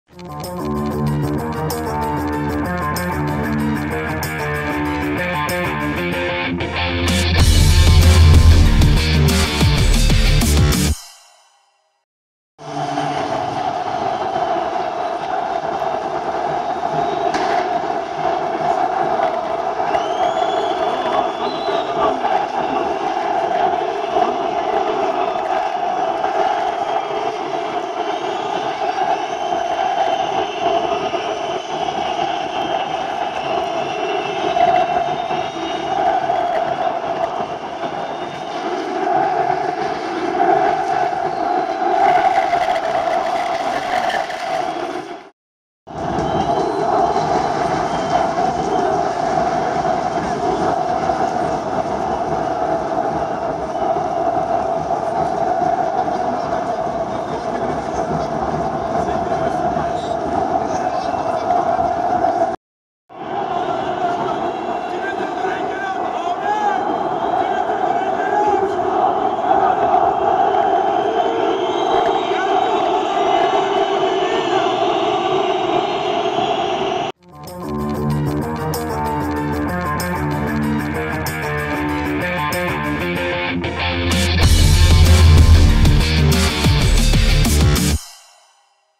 Bursapor taraftarı yeni sezonda ateş ediyor | Futbolun İçindekiler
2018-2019 Spor Toto Süper Lig'in başlamasının ardından Bursaspor taraftarı adeta Kadıköy'e çıkarma yaptı. 90 dakika susmayan yeşil-beyazlı taraftarlar takımlarını susmadan maç boyunca destekledi

Bursaspor
Teksas
Bursaspor Taraftar
Bursaspor show